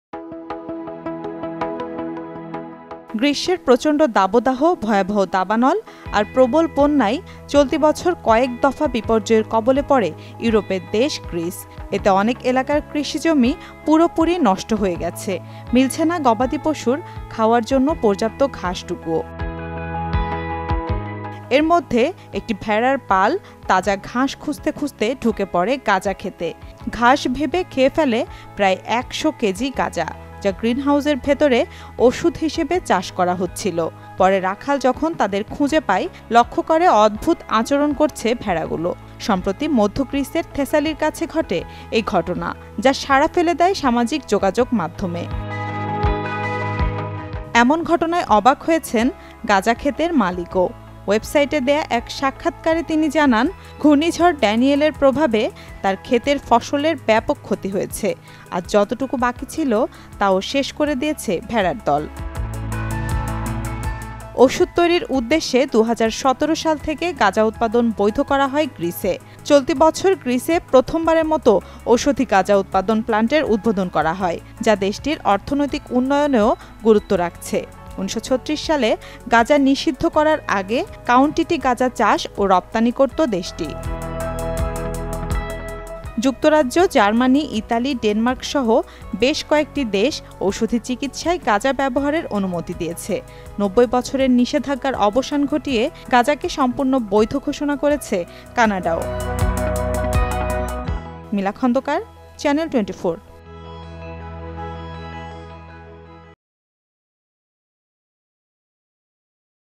গাঁজা ক্ষেত খেয়ে সাফ করলো ভেড়ার দল | Greece | Sheep | Marijuana | Channel 24
Herd Of Sheep Accidentally Eats 100 Kg Of Cannabis In Greece.

»» Office Address:
Bangladesh.